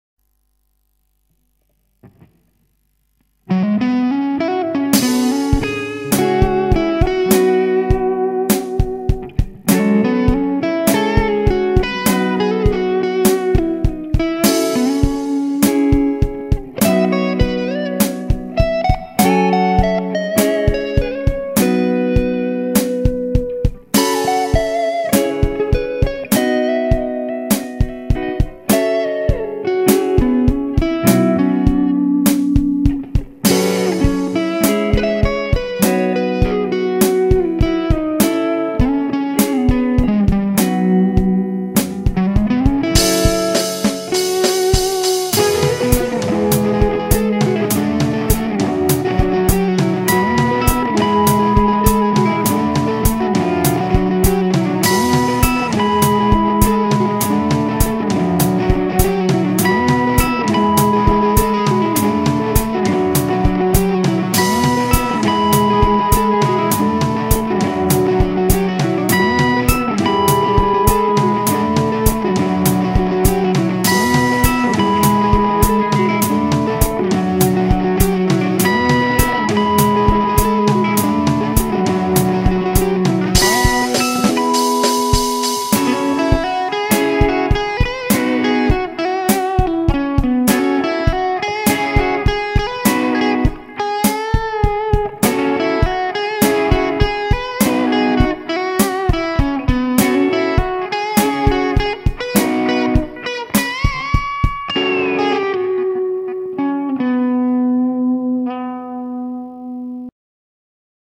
sentyment
Fotki znajomej, do tego improwizowana muzyka